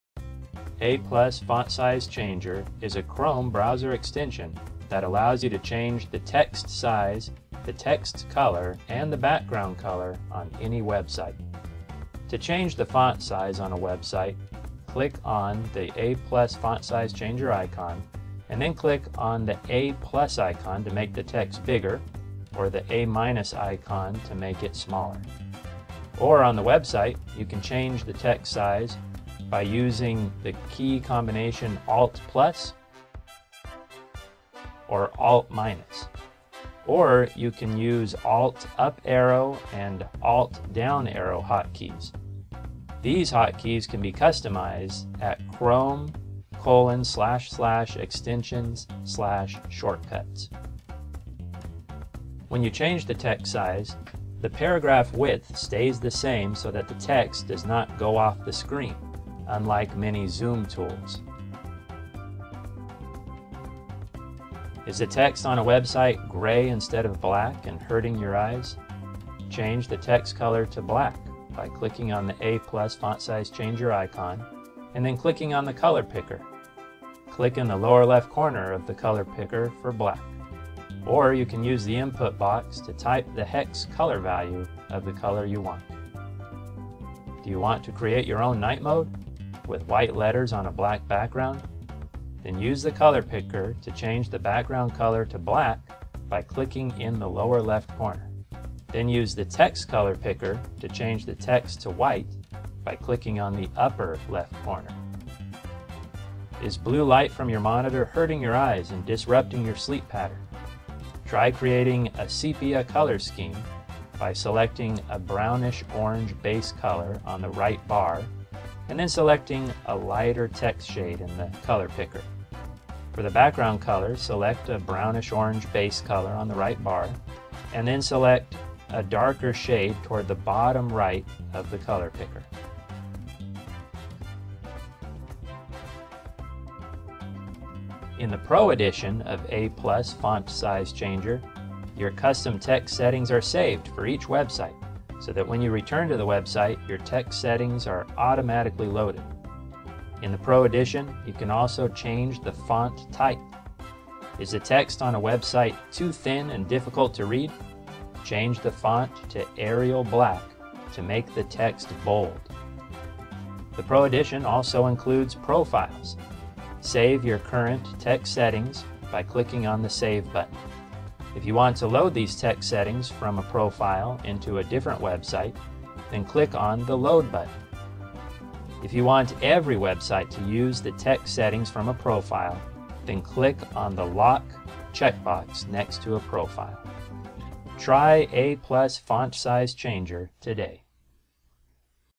A+ Font Size Changer Chrome Extension
A+ FontSize Changer is a Chrome browser extension that allows you to change the font text size, text color and background color on any website.

To change the font size on a website click on A+ FontSize Changer icon and then click on the "A+" icon to make the text bigger or click on the "A-" icon to make it smaller. Or on the website you can change text size by using the ALT+ or ALT- hotkeys. Or you can use ALT+Up Arrow or ALT+Down Arrow hotkeys. These hotkeys can be customized at chrome://extensions/shortcuts

When you change the text size, the paragraph width stays the same so that the text does not go off the screen, unlike many zoom tools.

Is the text on a website gray instead of black and hurting your eyes? Change the text color to black by clicking on the A+ FontSize Changer icon and then clicking on the color picker. Click in the lower left corner of the color picker for black. Or you can use the input box to type the hex color value of the color you want.

Do you want to create your own Night Mode with white letters on a black background? Then use the color picker to change the background color to black by clicking in the lower left corner. Then use the text color picker to change the text to white by clicking on the upper left corner.

Is blue light from your monitor hurting your eyes and disrupting your sleep pattern. Try creating a Sepia color scheme by selecting a brownish orange base color on the right bar and then selecting a lighter text shade. For the background color, select a brownish orange base color on the right bar and then select a darker shade toward the bottom right of the color picker.

In the Pro edition of A+ FontSize Changer your custom text settings are saved for each website so that when you return to the website your text settings are automatically loaded. In the Pro edition you can also change the font type. Is the text on a website too thin and difficult to read? Change the font to Arial Black to make the text bold.

The Pro edition also includes Profiles. Save your current text settings by clicking on the "Save" button. If you want to load the text settings from a Profile into a different website then click on the "Load" button. If you want every website to use the text settings from a profile then click on the "Lock" checkbox next to a profile.

Try A+ FontSize Changer today!